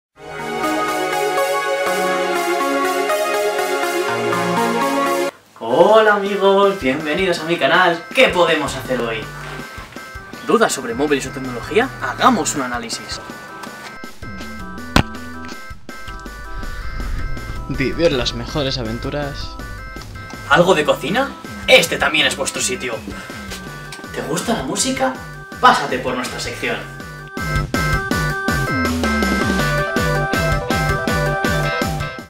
BIENVENIDOS A MI CANAL - 2015!
Hoooooola amigos, bienvenidos de nuevo a mi canal! Aquí os traigo (POR FIN), una intro un poquillo más actualizada que la anterior!
Si queréis explorar pasaros por el resto de mis videos, al final os dejo unos cuantos!
Muchas gracias! Y a seguir disfrutando! Un saludo!